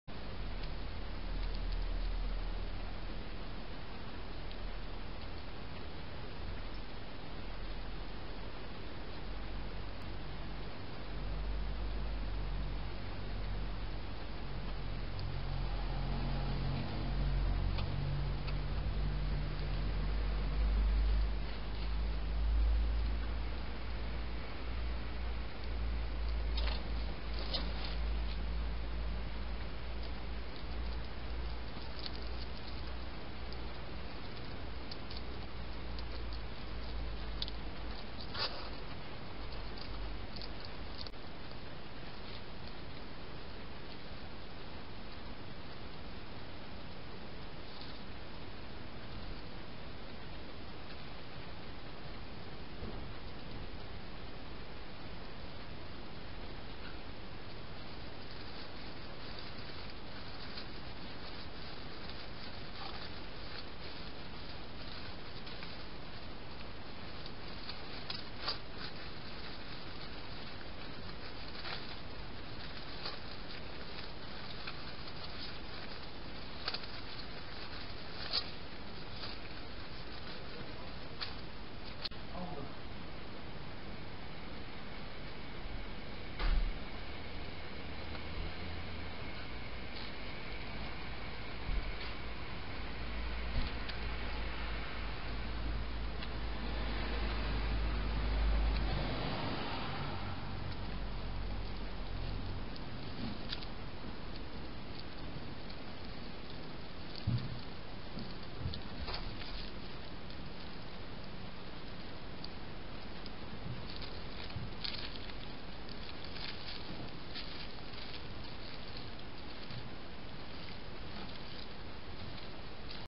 2 Babies!😀😀
2 out of 4 babies having a little explore outside the nest!